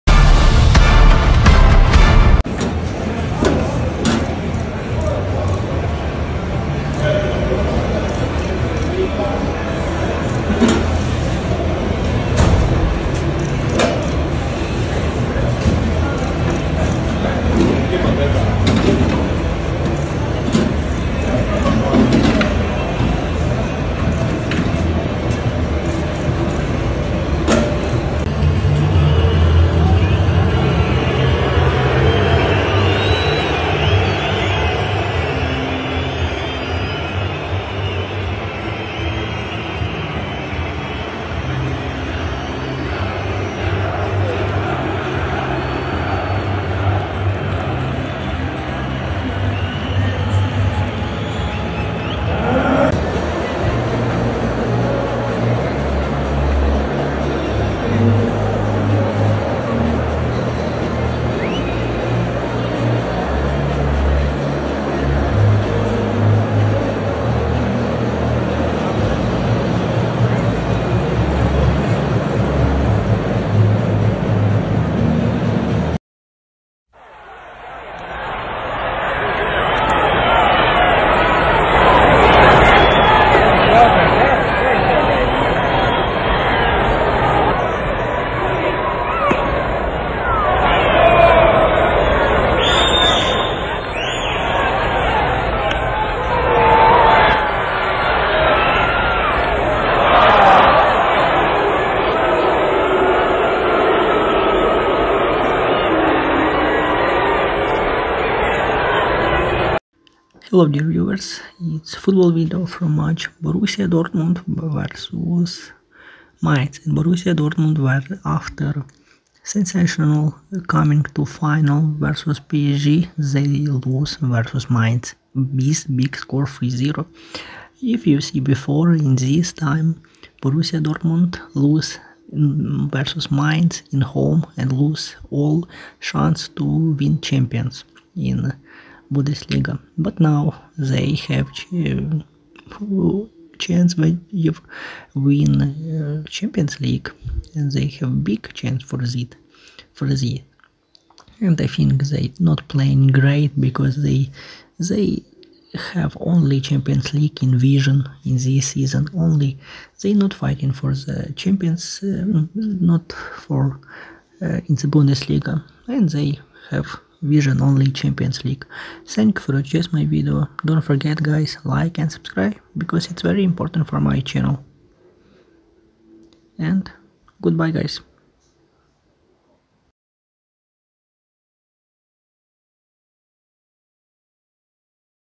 1. FSV Mainz 05 - Borussia Dortmund 3-0 Zusammenfassung | Bundesliga 2023-2024
1. FSV Mainz 05 - Borussia Dortmund